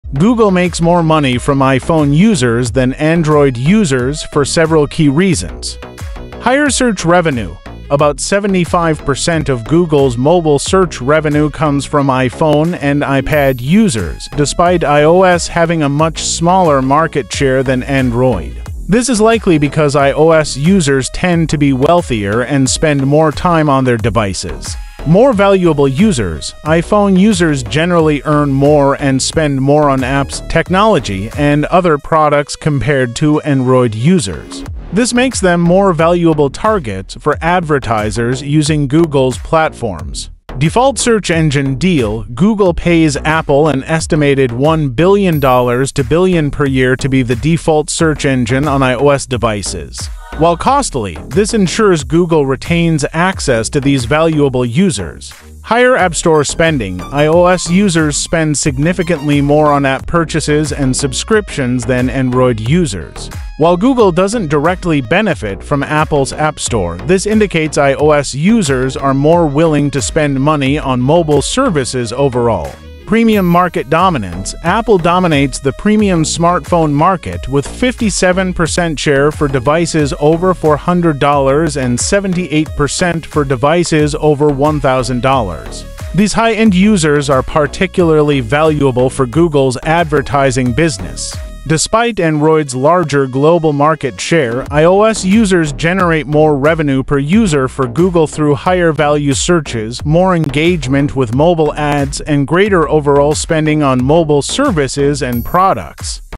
Why Google Makes More From iPhone Users Than Android Users
Google makes more money from iPhone users than Android users for several key reasons:

1. Higher search revenue: About 75% of Google's mobile search revenue comes from iPhone and iPad users, despite iOS having a much smaller market share than Android[3]. This is likely because iOS users tend to be wealthier and spend more time on their devices[3].

2. More valuable users: iPhone users generally earn more and spend more on apps, technology, and other products compared to Android users[1][4]. This makes them more valuable targets for advertisers using Google's platforms.

3. Default search engine deal: Google pays Apple an estimated $1-2 billion per year to be the default search engine on iOS devices[3]. While costly, this ensures Google retains access to these valuable users.

4. Higher app store spending: iOS users spend significantly more on app purchases and subscriptions than Android users[4]. While Google doesn't directly benefit from Apple's App Store, this indicates iOS users are more willing to spend money on mobile services overall.

5. Premium market dominance: Apple dominates the premium smartphone market, with 57% share for devices over $400 and 78% for devices over $1000[1][4]. These high-end users are particularly valuable for Google's advertising business.

Despite Android's larger global market share, iOS users generate more revenue per user for Google through higher-value searches, more engagement with mobile ads, and greater overall spending on mobile services and products[3][4].

[2] ELI5: How does Apple make more money than Google / Android?
[3] Why iOS Is More Valuable to Google Inc. Than Android
[4] iPhone Vs. Android Users 2023: What Your Business Needs to Know
[5] Google Makes over $92B per Year by Owning Android - Hacker News